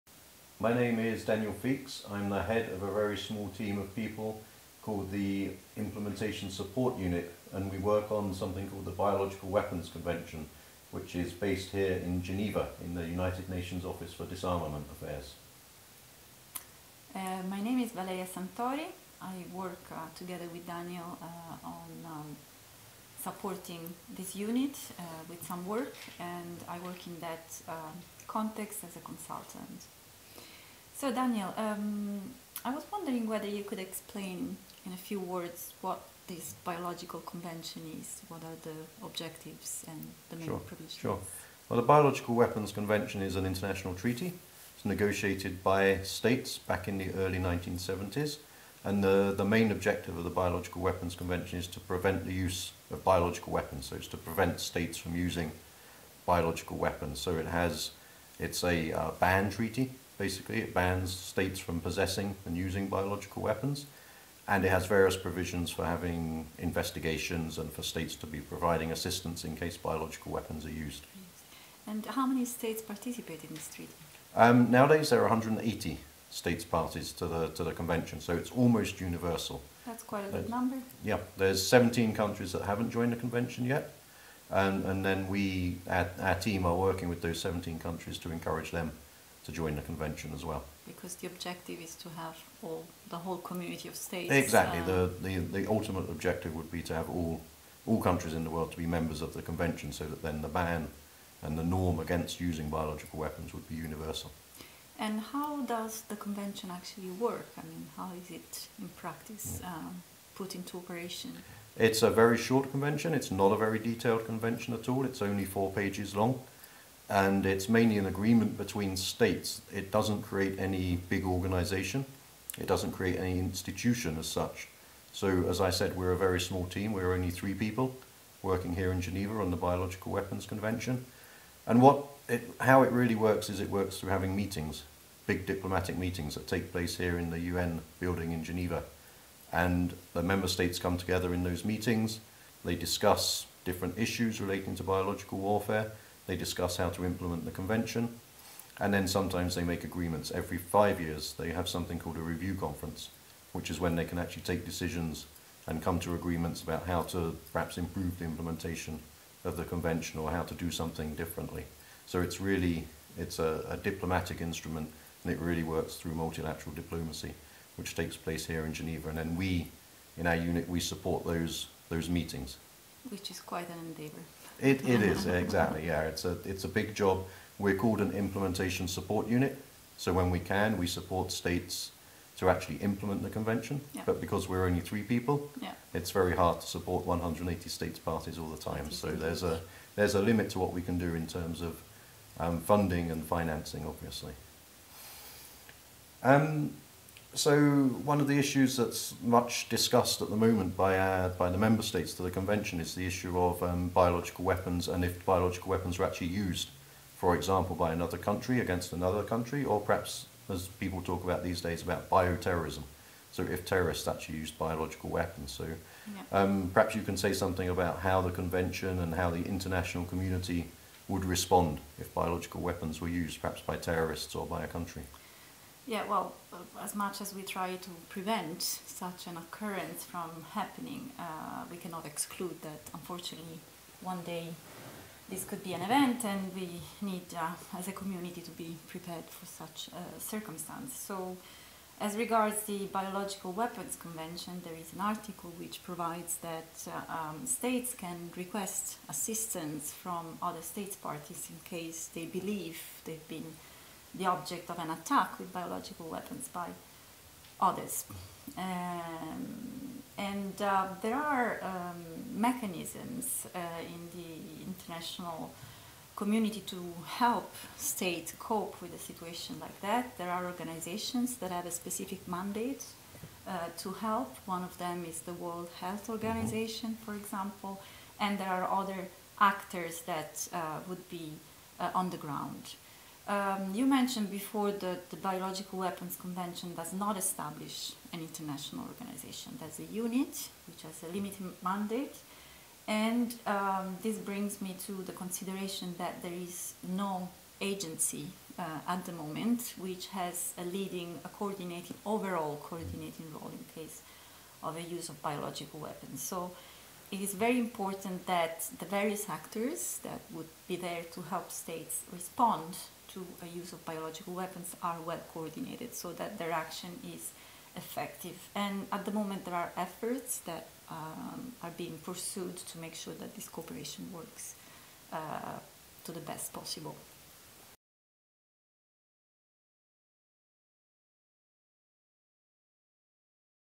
GCSP: What is the Biological Weapons Convention?
Mr Daniel Feakes, Head of the Implementation Support Unit (ISU) for the Biological Weapons Convention, United Nations Office for Disarmament Affairs in Geneva, and Ms Valeria Santori, consultant at the ISU for the BWC, discuss the key objectives, State Party roles and implementation strategies of the Biological Weapons Convention.